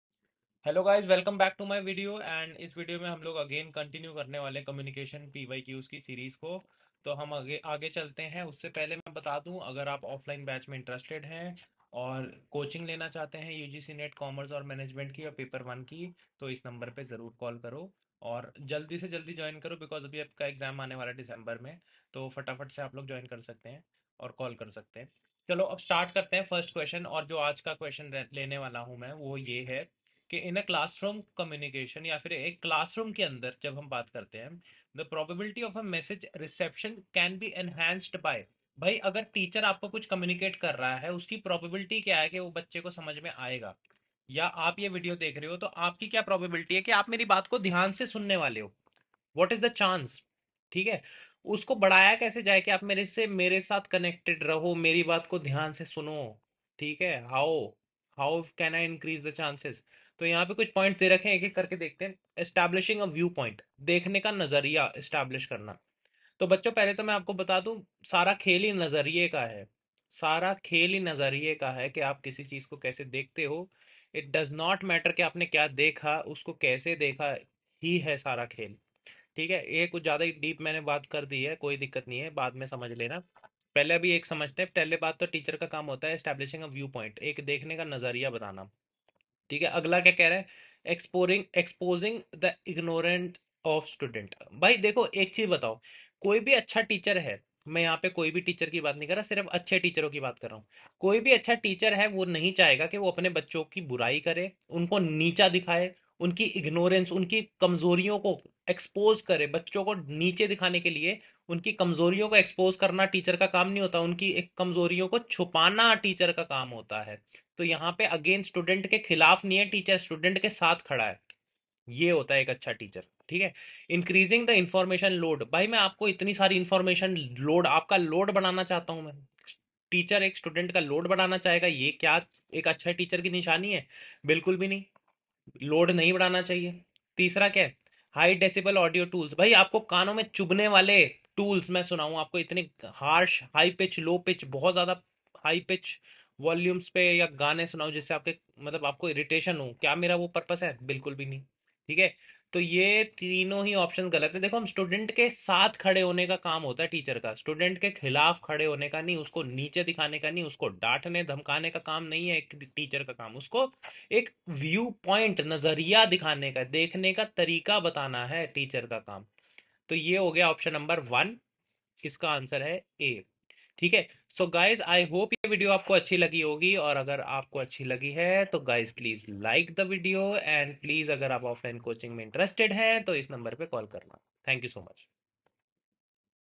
5. In a classroom, the probability of message reception can be enhanced by ? UGC NET PYQ's Commerce
Q1. In a classroom, the probability of message reception can be enhanced by

(1) Establishing a viewpoint

(2) Exposing the ignorance of students

(3) Increasing the information load

(4) Using high-decibel audio tools